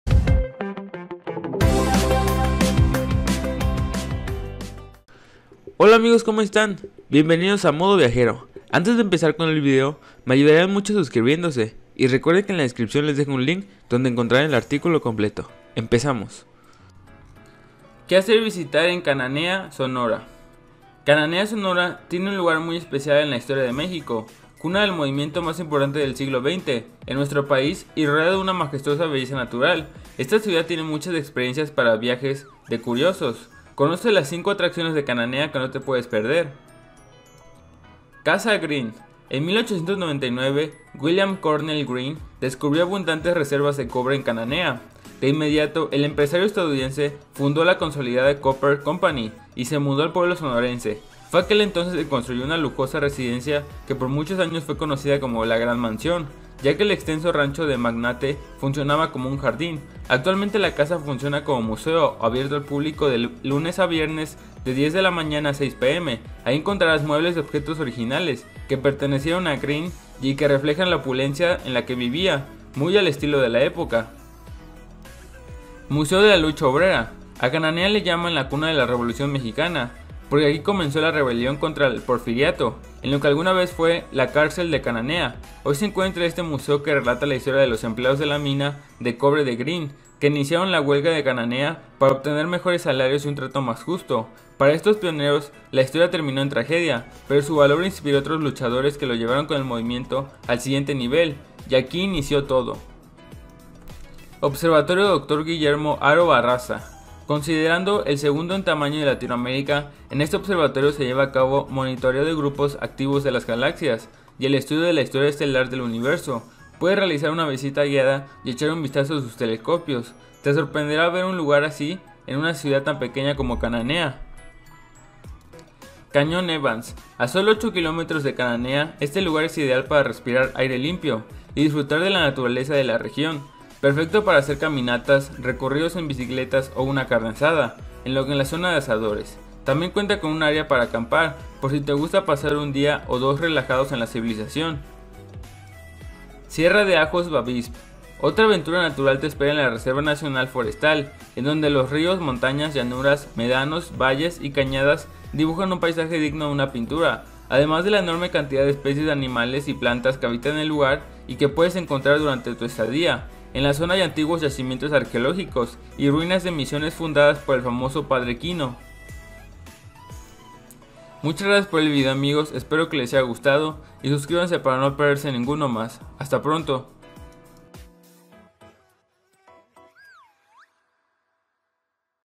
¿Qué hacer y visitar en Cananea Sonora? Lugares turísticos y actividades
00:00 Cananea Sonora
00:37 Casa Greene
01:14 Museo de la Lucha Obrera
01:43 Observatorio Dr. Guillermo Haro Barraza
02:05 Cañón Evans
02:25 Sierra de Ajos Bavispe
02:50 Final

2023 Cananea Sonora
Guia Completa Cananea Sonora
Visita Cananea Sonora
Atracciones imperdibles Cananea Sonora